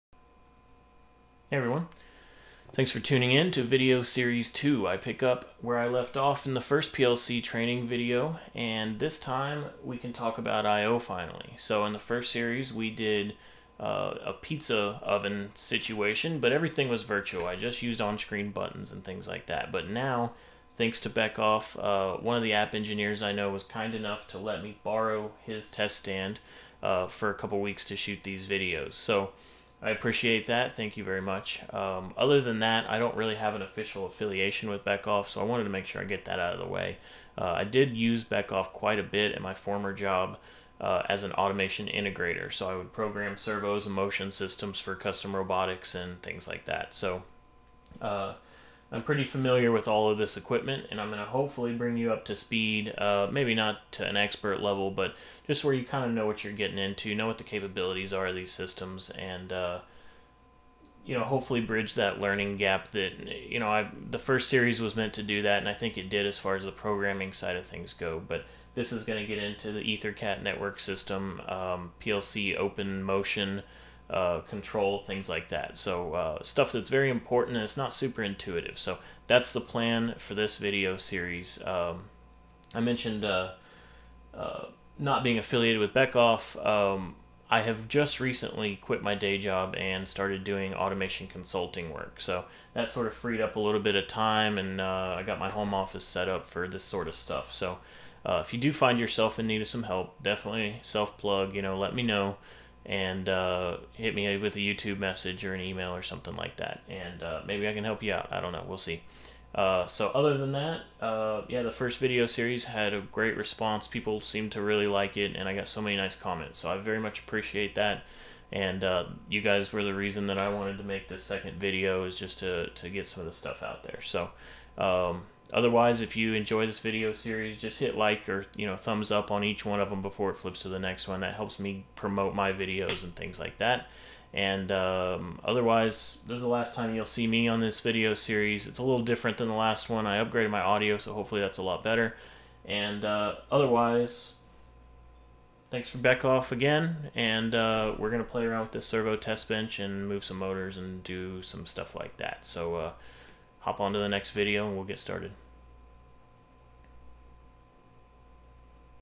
Motion Control and IO with PLCs - EP1 - Series Intro
Welcome to the next installment of 'learning PLCs...' from squishybrained.

On this series, I dive into motion control using Beckhoff TwinCAT, utilizing the PLCOpen motion library TC_MC2 from Beckhoff.

If you complete this series, you should end up with a good understanding of how to program stepper and servo systems in a TwinCAT PLC environment, as well as having a good foundation for learning other PLCOpen based systems.